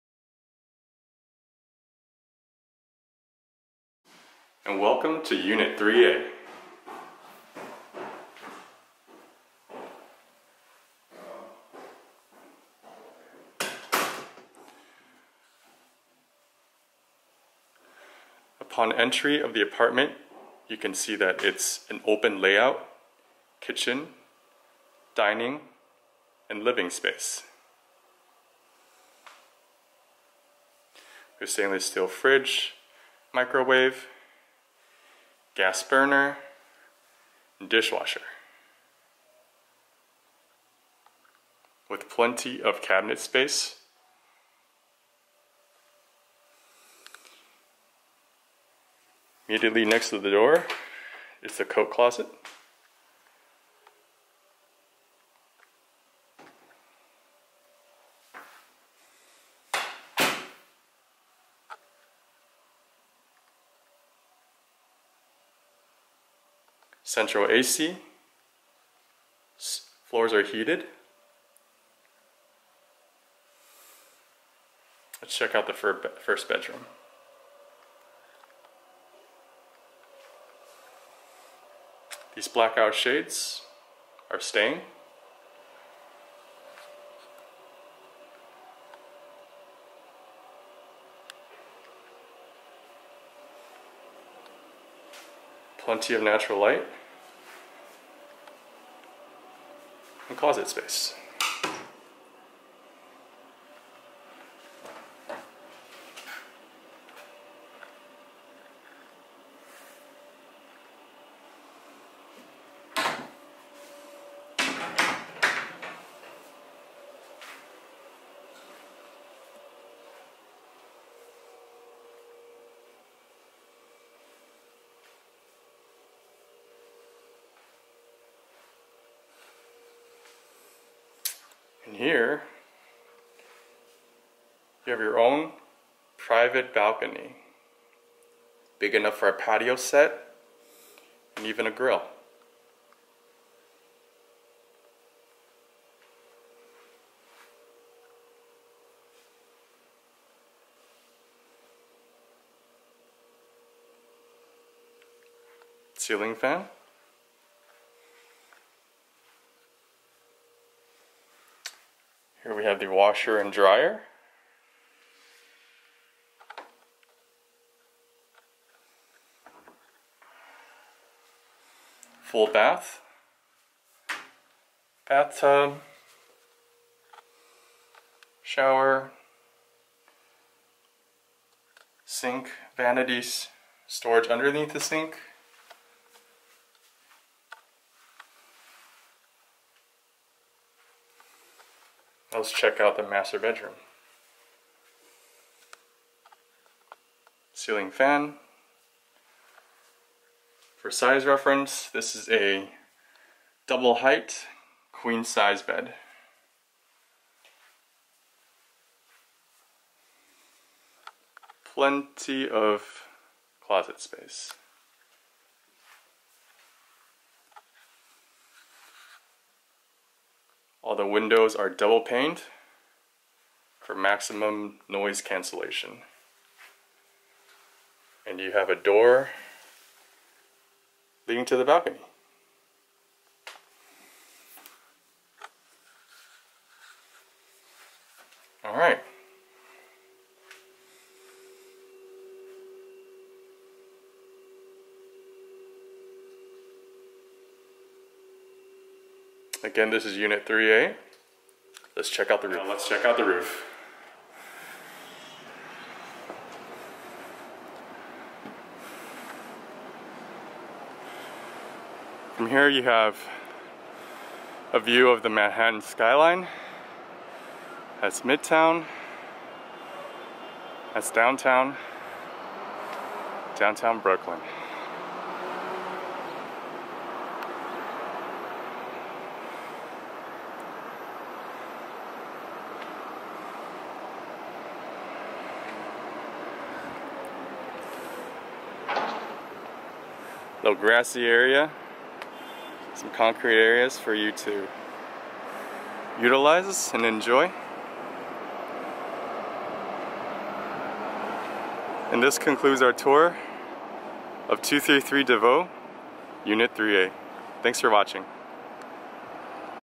2 bed 1 bath condo in Williamsburg
Bright and airy 2-bedroom condo on a quiet block in East Williamsburg. Large open concept living / dining space leads to private balcony. The master bedroom is queen-sized and the second bedroom is full sized. Both bedroom have built-in closet space. The bathroom is a full-sized with a bathtub. Kitchen includes stainless steel appliances, gas stove & oven, microwave, dishwasher, and Caesarstone countertop.

Radiant heat, stone tile flooring throughout. High efficiency, central heat and air conditioning. Soundproof Thermopane windows. Washer & dryer in-unit.

The LEED-certified green building has solar panels that power the condominium's common areas and bike room. Shared rooftop provides amazing views of the Manhattan skyline. Convenient walk to both Graham and Grand stops (L train). Pets case-by-case.